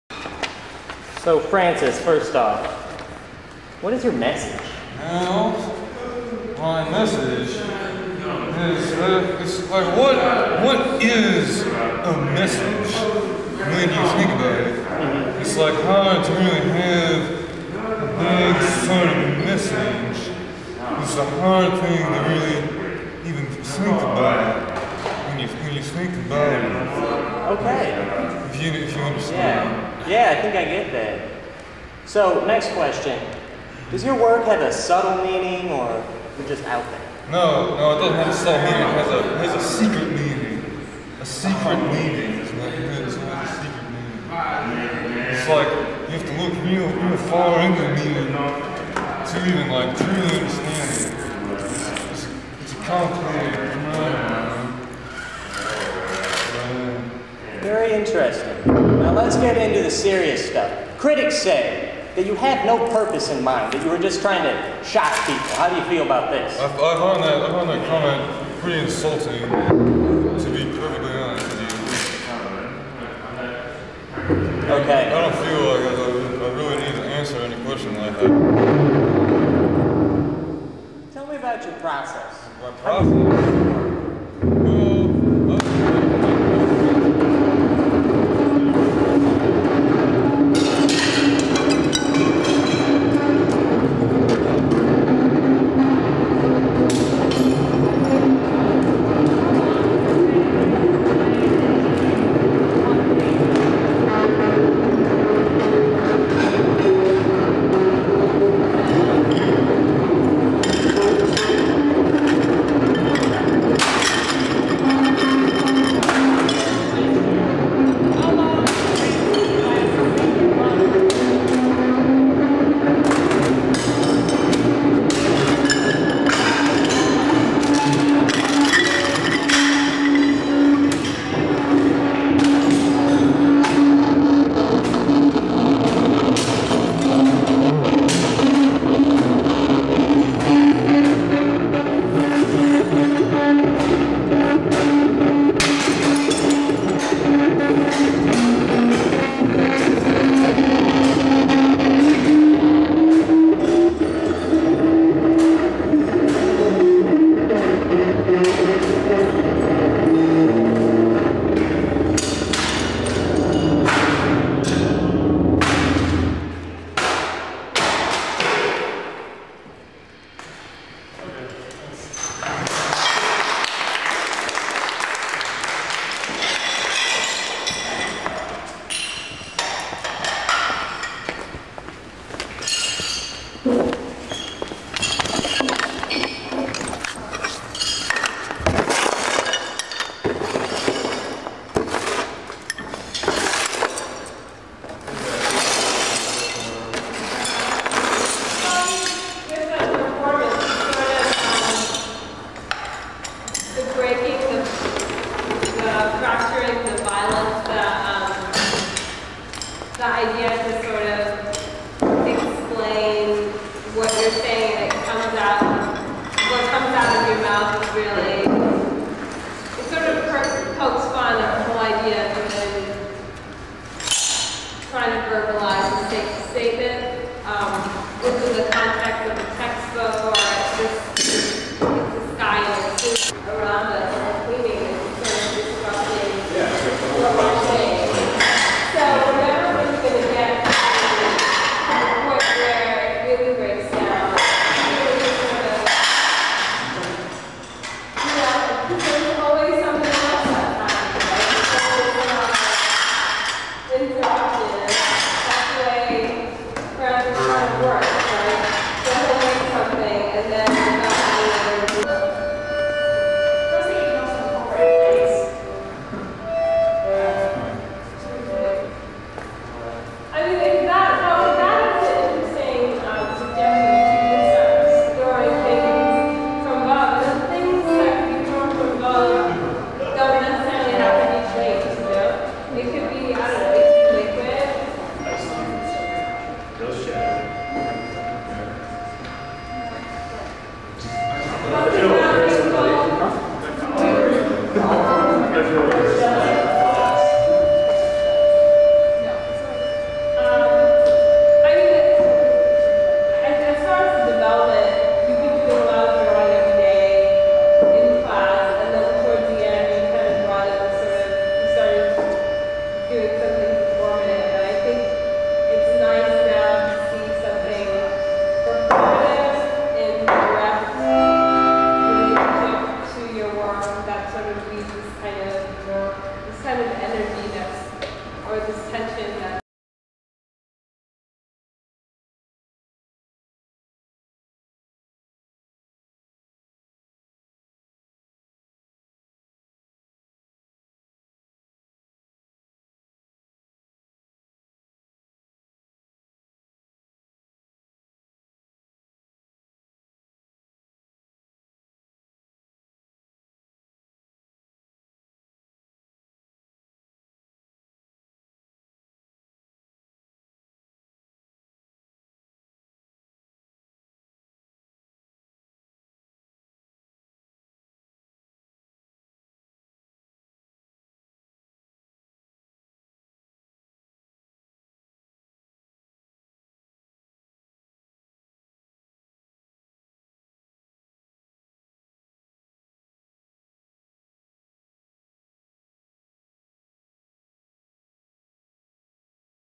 Artist Statement as 3 Bob Dylans
Francis Zaander's artist statement presented as three Bob Dylans